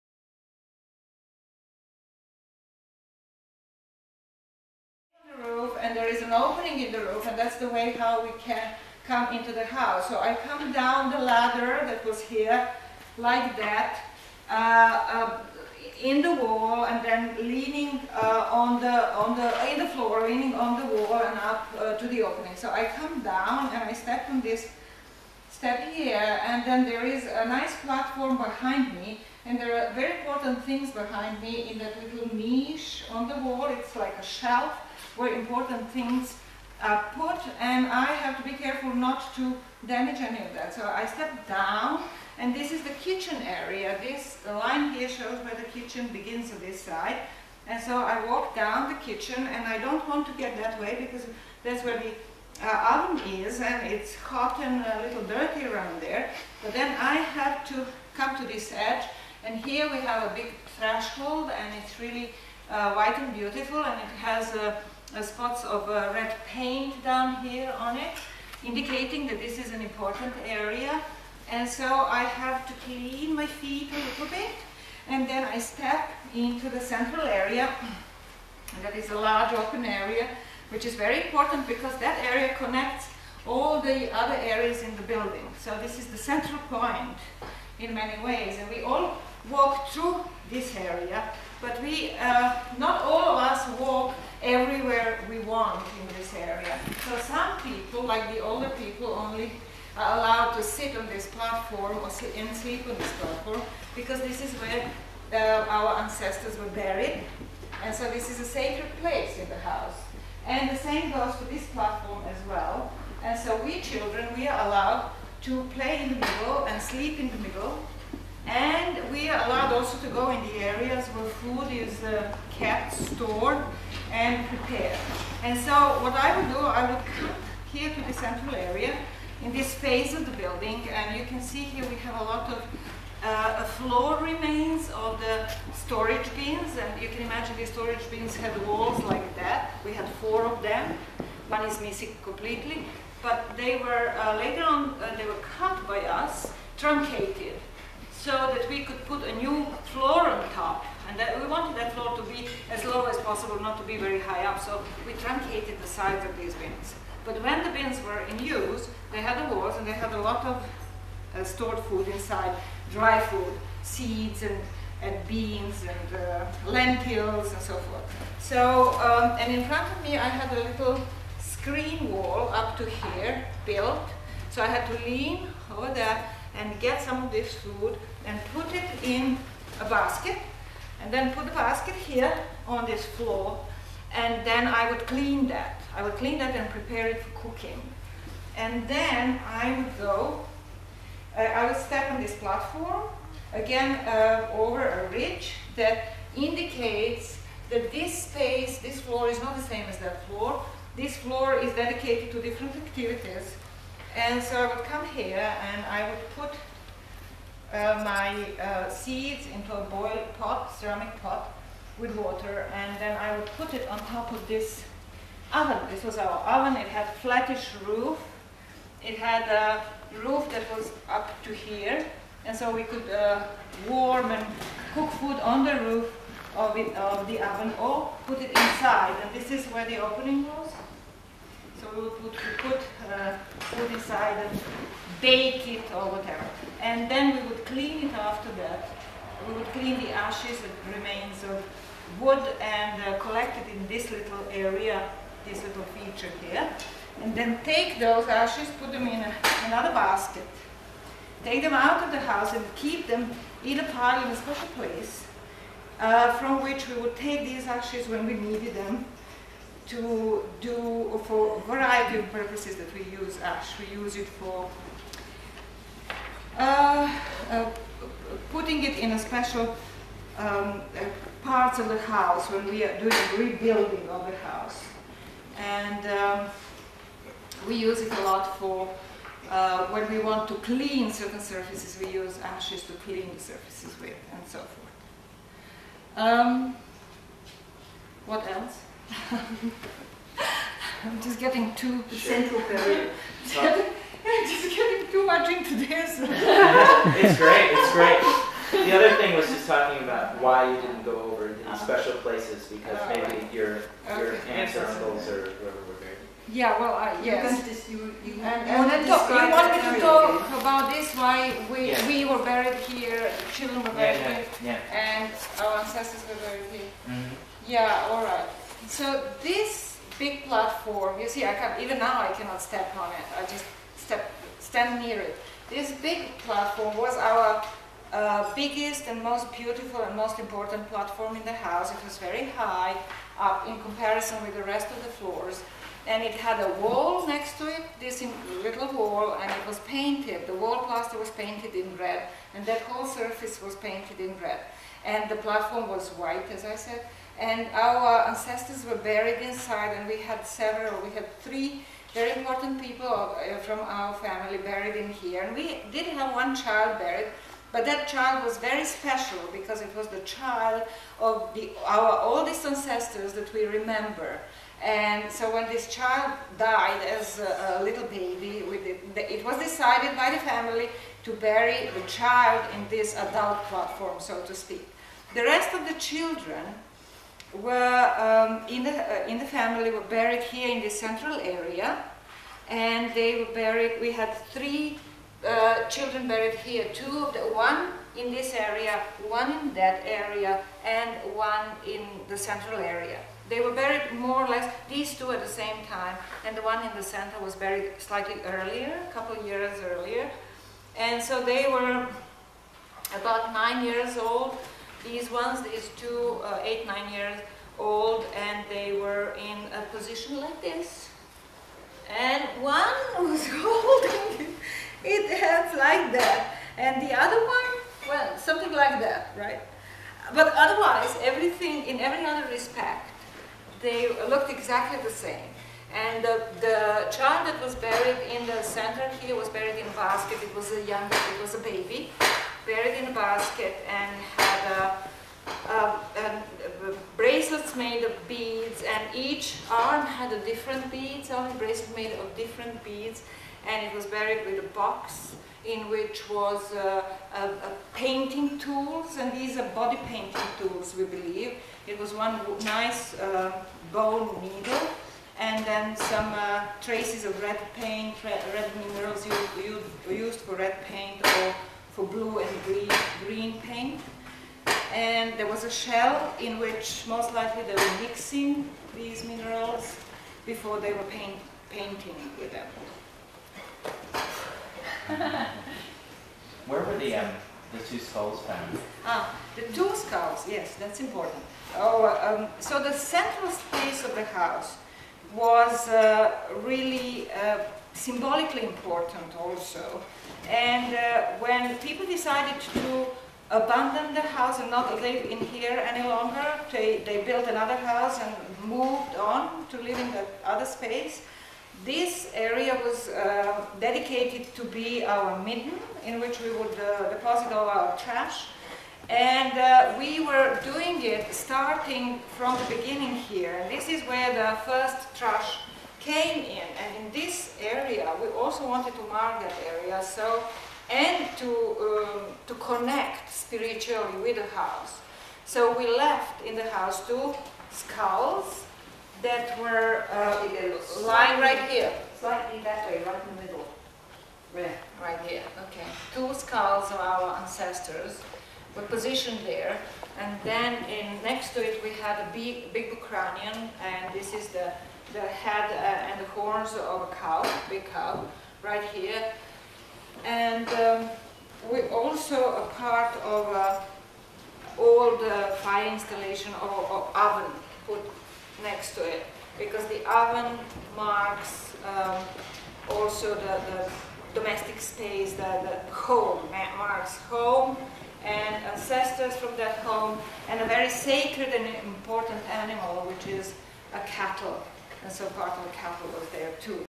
June13, 2002: Mira's Neolithic Tour of Building 3
This is the video record of a tour of Building 3 and what it was like to live in and move around the building during the Neolithic 9000 years ago, given on 13 June, 2002 in the BACH Area of Çatalhöyük by Mirjana Stevanovic. videographer:Michael Ashley